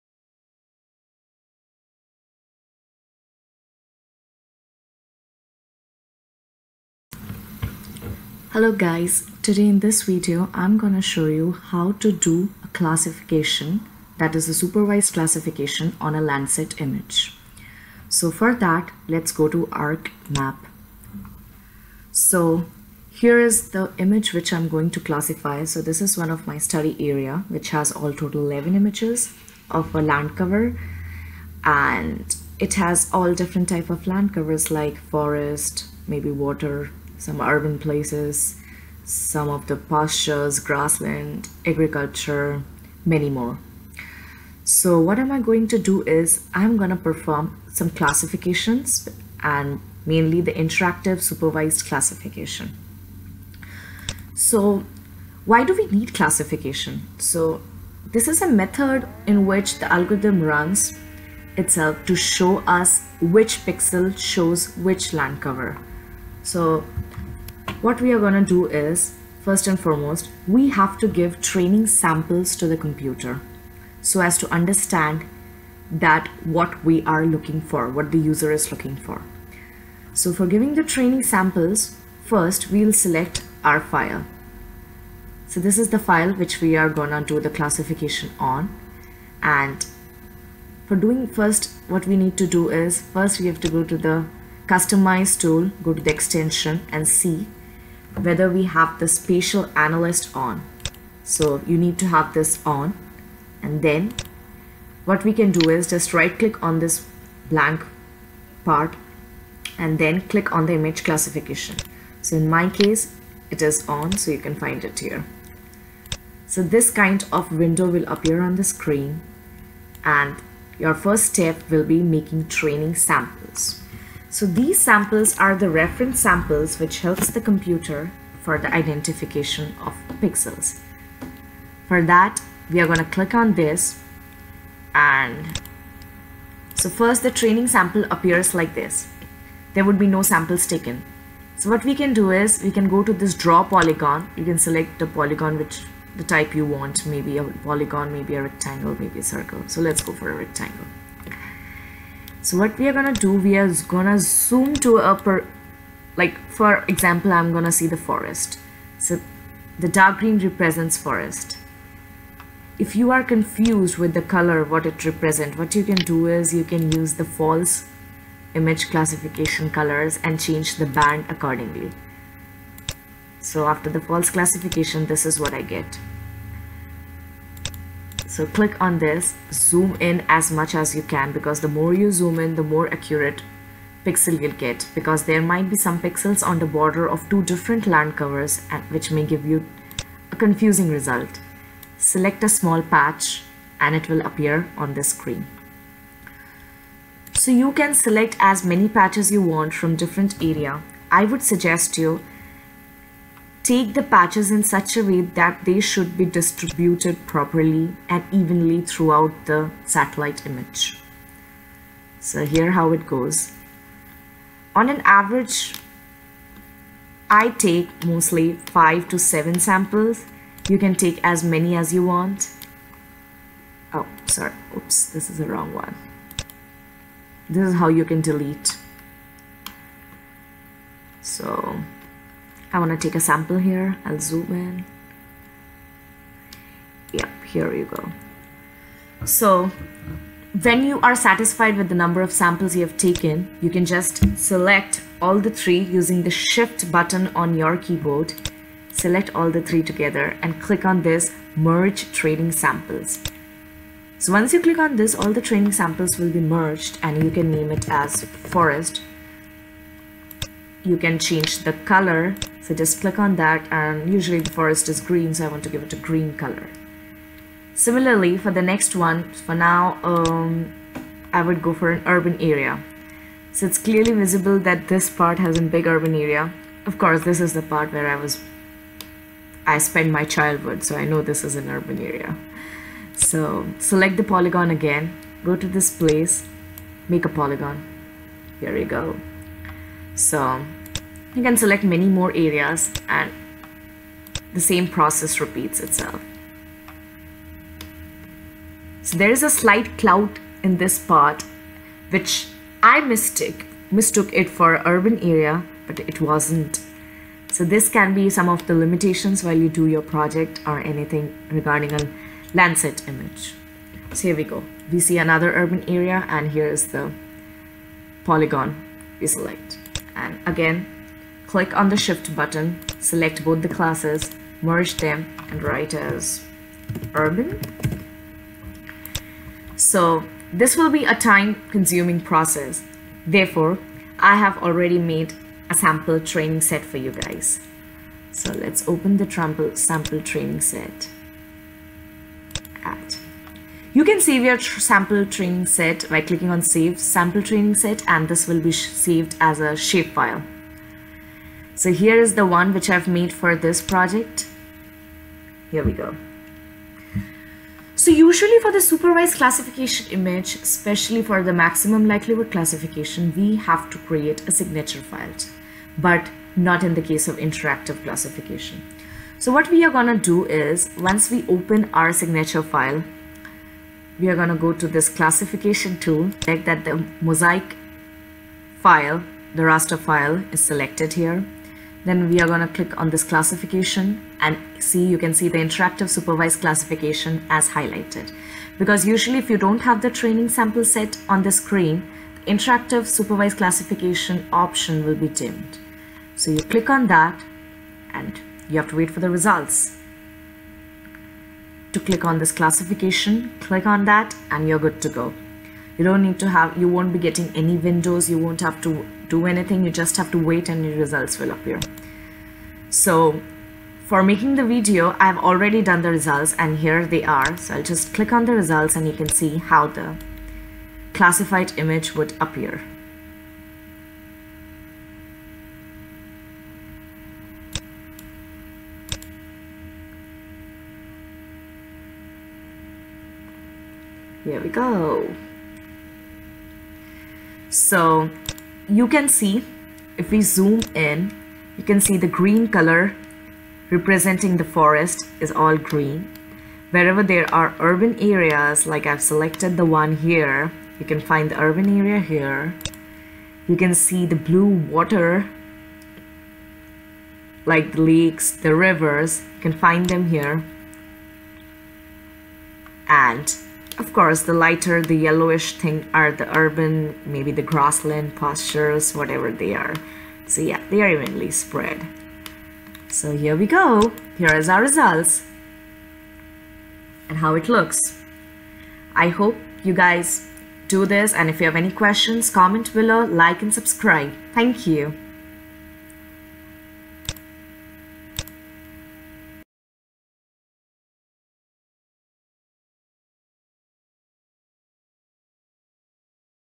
Interactive supervised classification using ArcGIS 10.4.1
This video explains about the one of the supervised classification using ArcGIS 10.4.1.